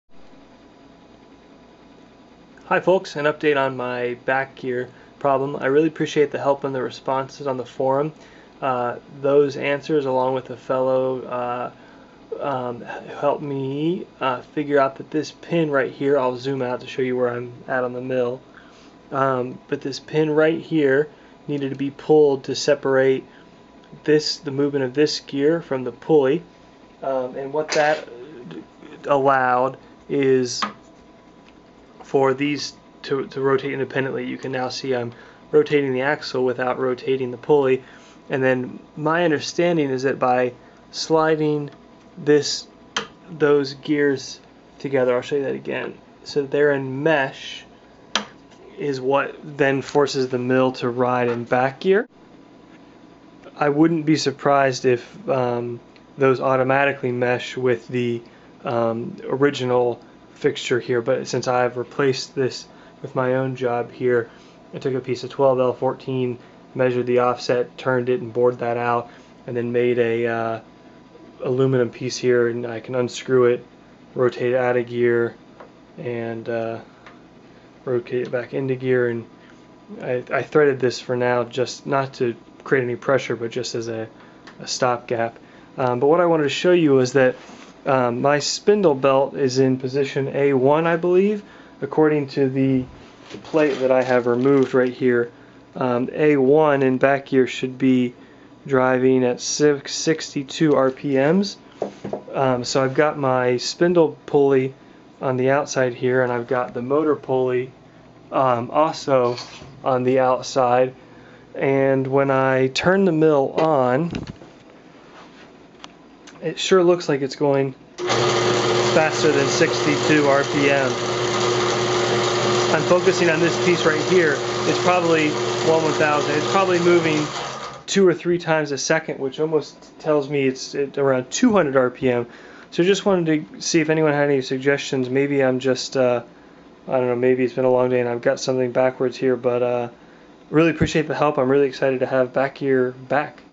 Atlas MFC Mill Back Gear - Follow Up
Fixing my back gear on my atlas MFC mill 5 Reasons to Use a Fixture Plate on Your CNC Machine